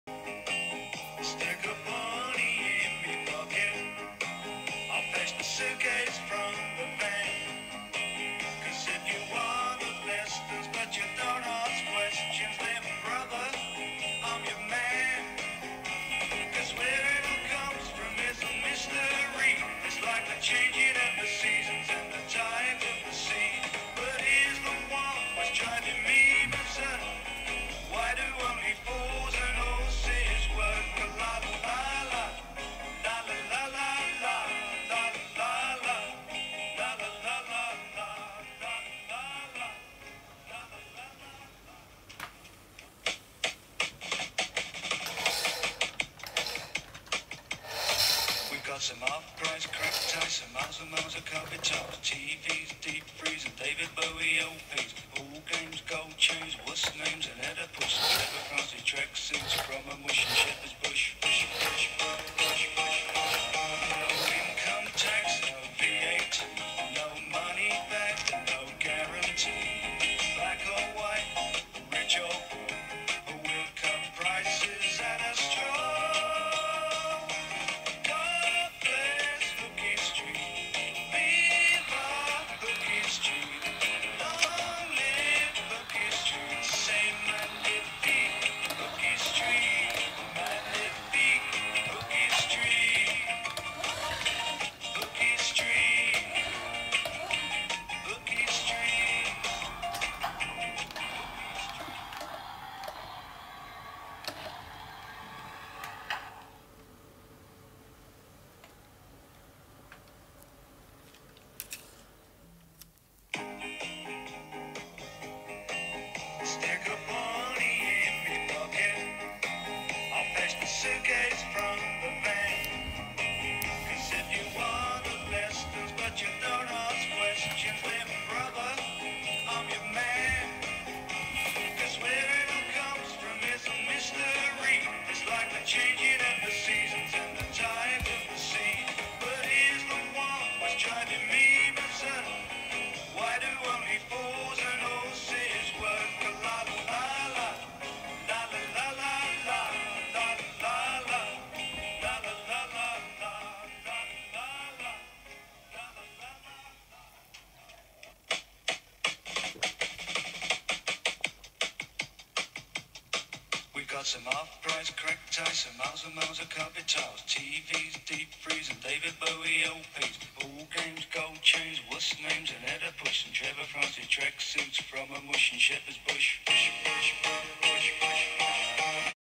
Prat-mobile’ car loved by Only Fools and Horses Del Boy Ford Capri & van remote control cars
“Prat-mobile’ car loved by Only Fools and Horses Del Boy, ford Capri & reliant robin
Remote controlled Capri & Van